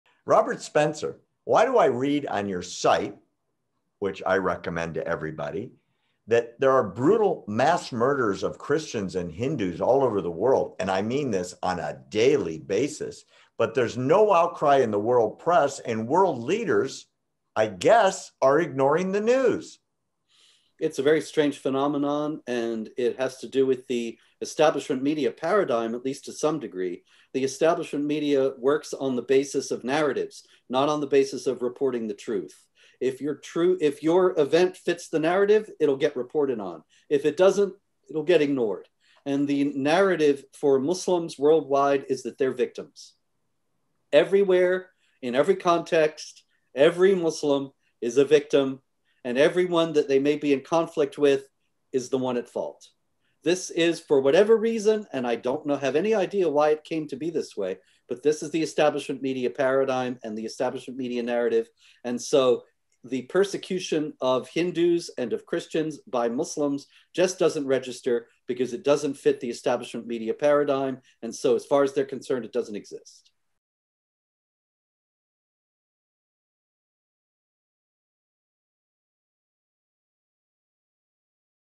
Why the Media Ignores Islamic Jihad Violence Against Christians and Hindus
Jihad Watch director Robert Spencer discusses why the establishment media so often fails to report on Muslim violence against Christians and Hindus. With Barry Nussbaum of the American Truth Project.